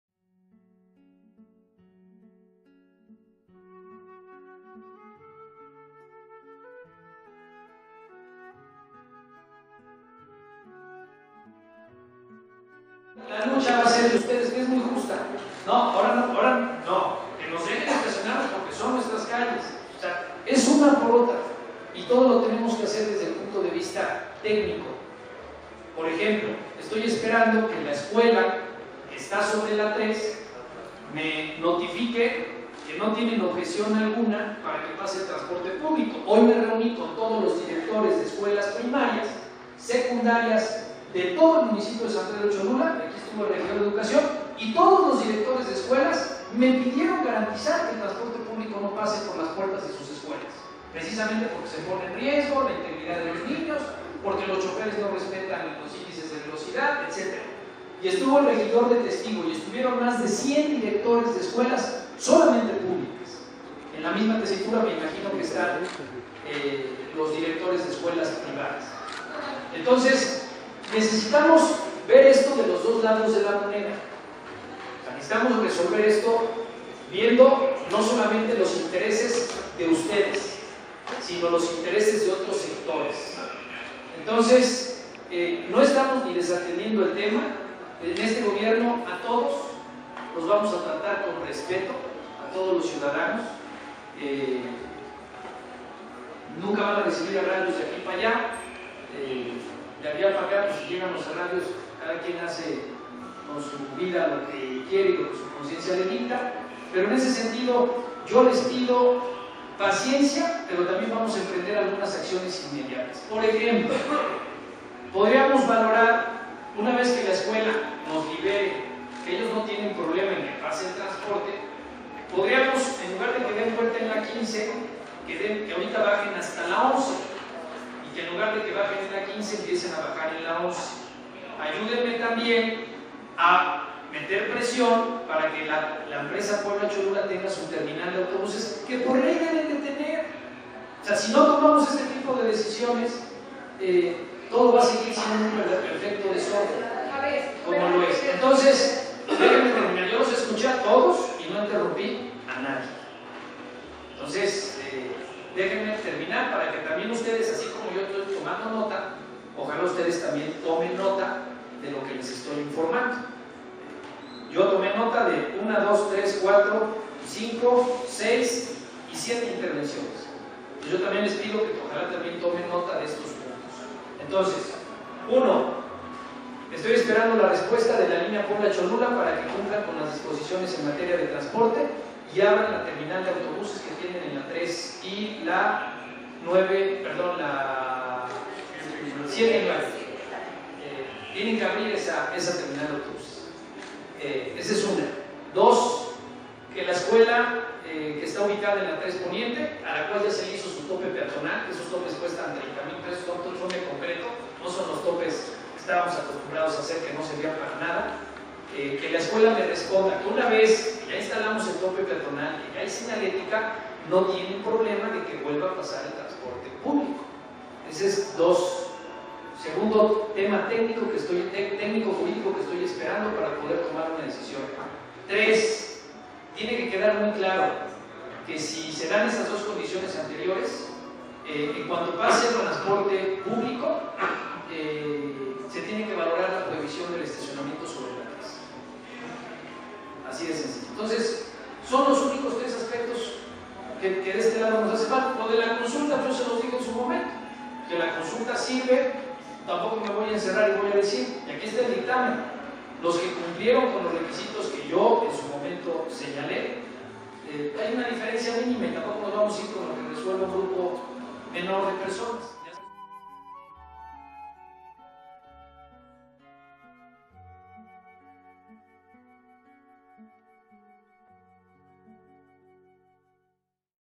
Debe ser una por una
Debe ser una por una, dice José Juan Espinosa, edil de San Pedro Cholula, a vecinos de la 3 Sur, si hay transporte colectivo, no habrá estacionamiento.